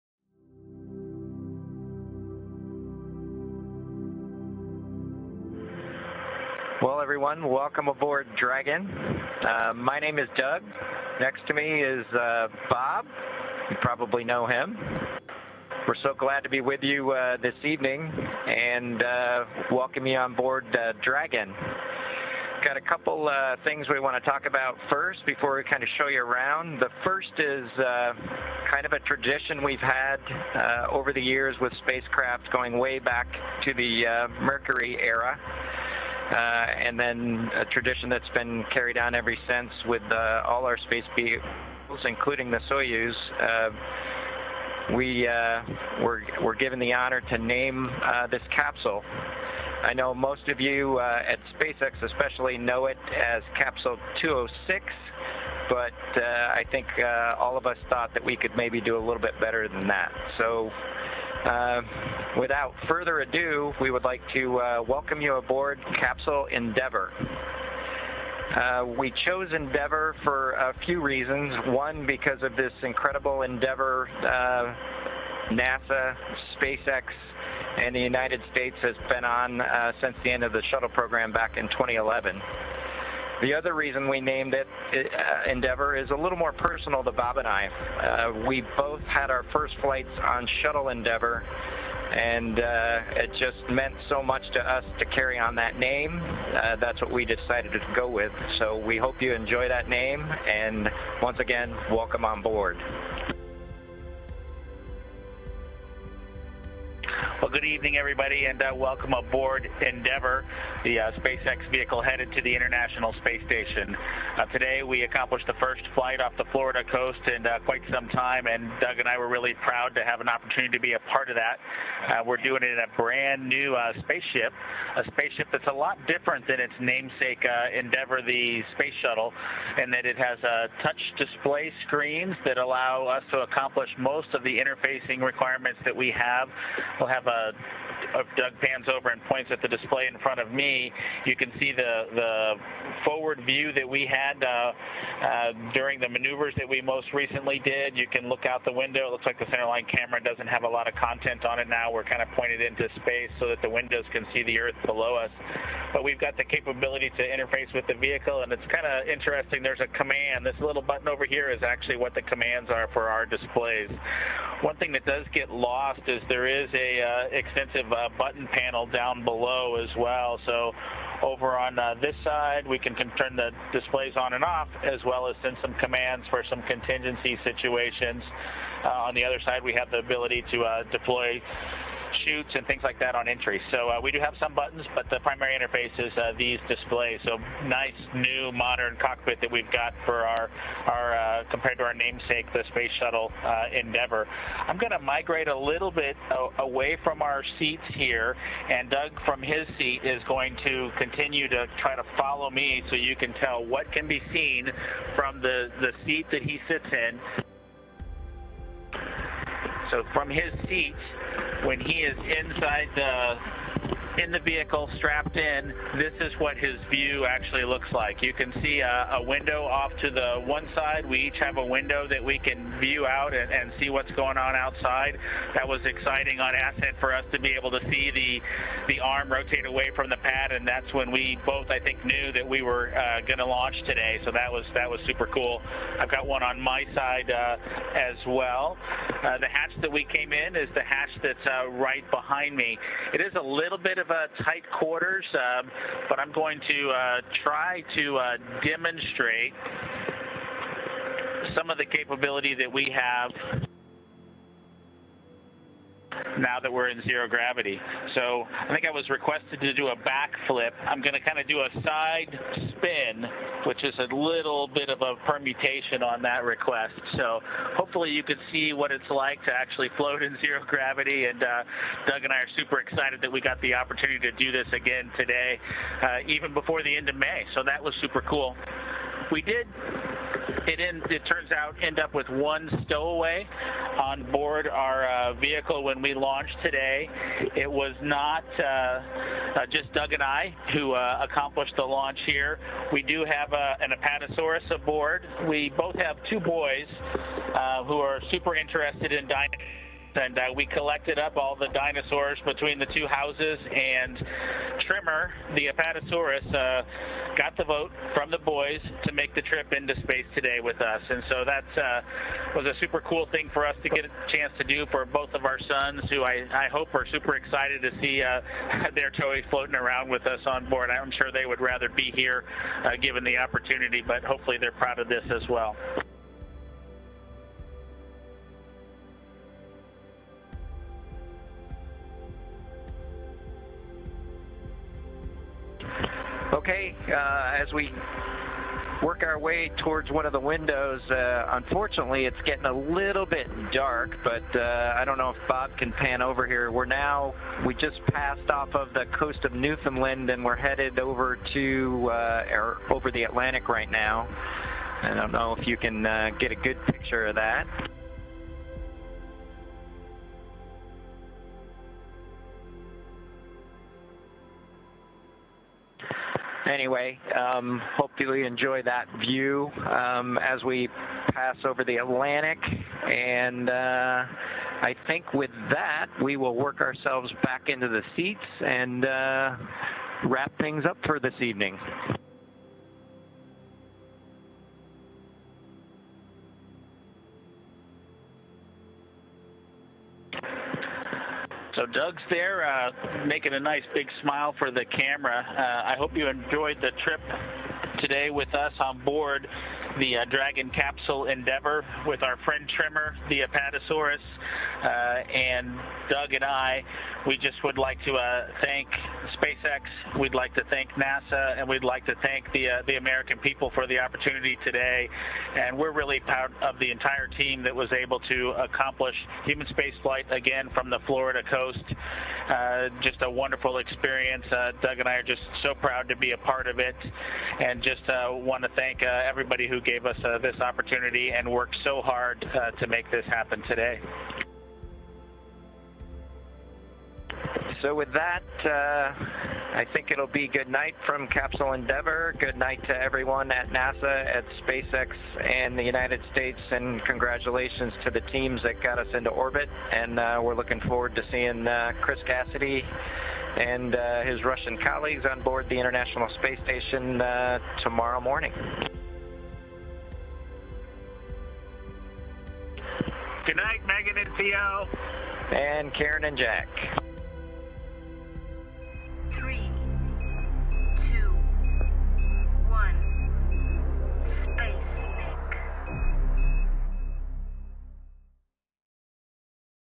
Doug and Bob in Space - Congratulations
Florida May 30, an American rocket launching from American soil, placing NASA astronauts Doug Hurley and Bob Behnken into orbit in the new Crew Dragon spacecraft for their journey to the International Space Station. Some 12 minutes after a spectacular liftoff from Launch Pad 39-A, Crew Dragon separated from the second stage of the Falcon 9 rocket and Hurley and Behnken began monitoring a series of test objectives for the duration of the vehicle’s 19-hour flight to the orbital outpost in the first crewed mission for the Commercial Crew Program. The veteran astronauts are scheduled to oversee an automated docking of Crew Dragon to the station May 31 to join NASA astronaut and Expedition 63 Commander Chris Cassidy of NASA and Russian crewmates Anatoly Ivanishin and Ivan Vagner